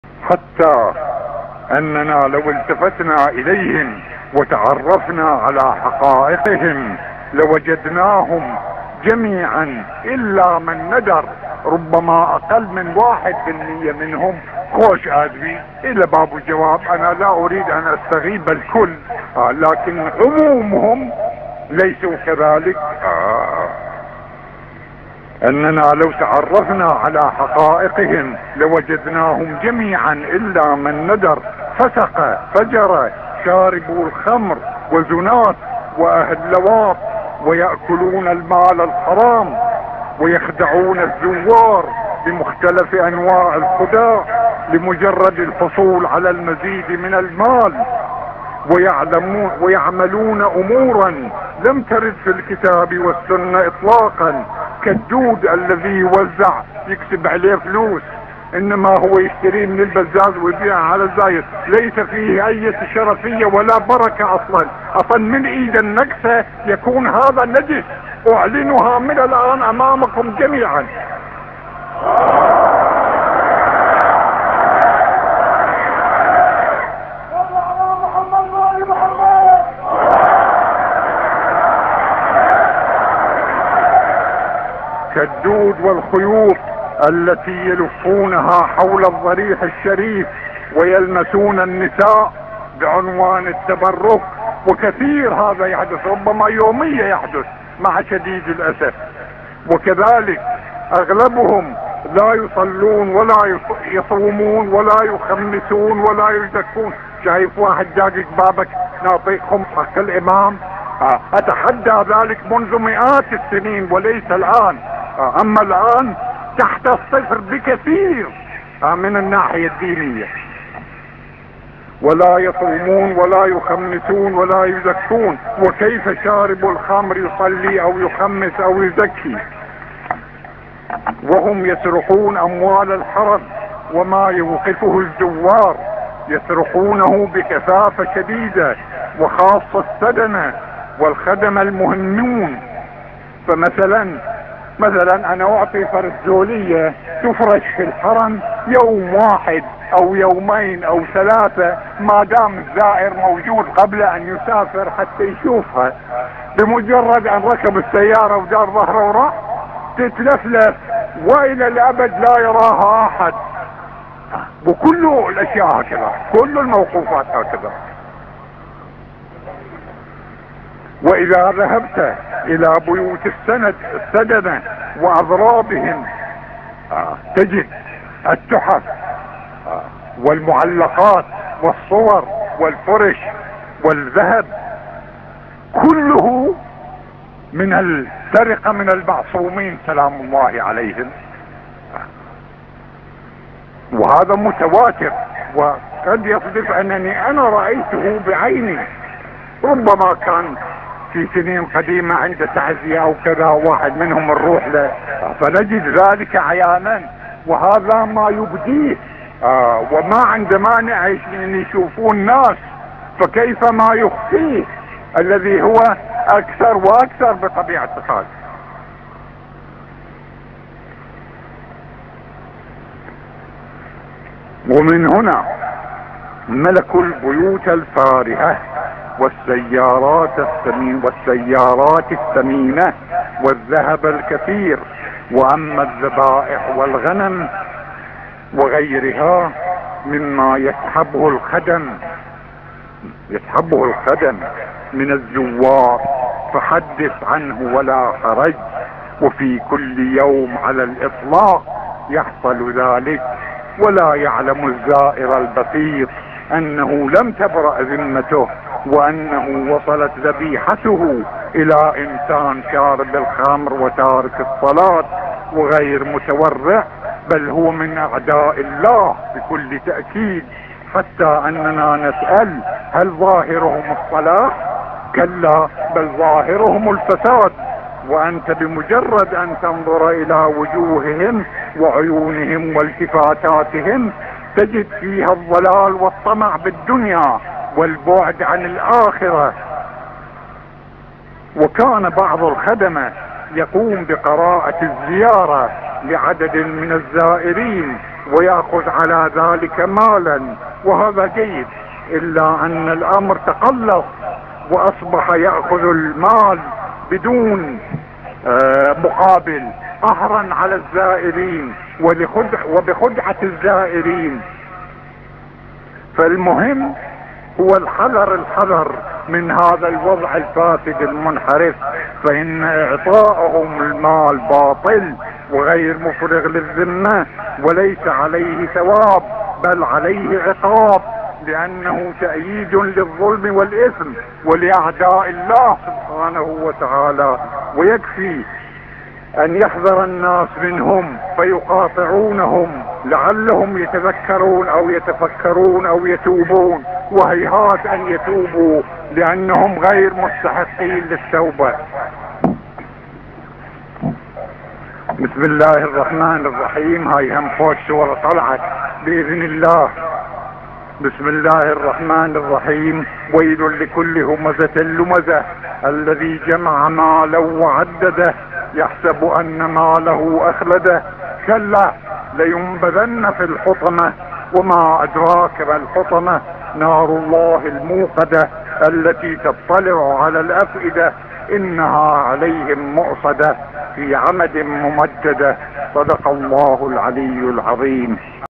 السيد الشهيد محمد الصدر يتحدث عن صفات السدنة ومتولي العتبات المقدسة وفسادهم
السيد الشهيد
الجمعة 16 - الخطبة الأولى